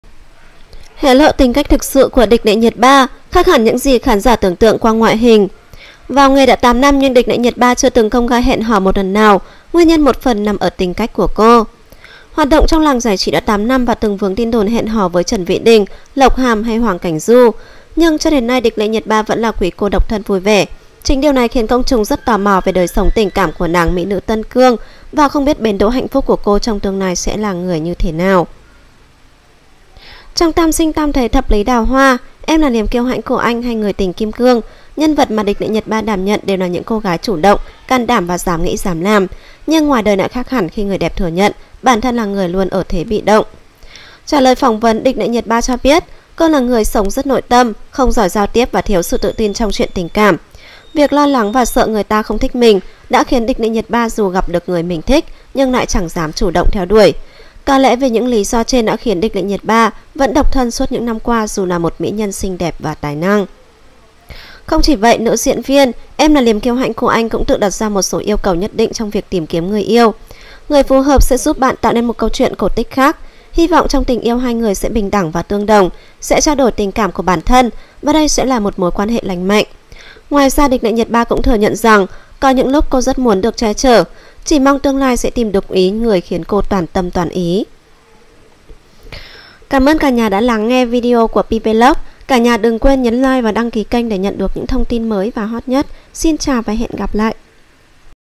ĐỊCH LỆ NHIỆT BA CHIA SẺ TIÊU CHUẨN TÌM KIẾM BẠN TRAI, LIỆU CÓ GIỐNG DƯƠNG DƯƠNG KHÔNG ĐÂY??
Nguồn thegioidienanhvn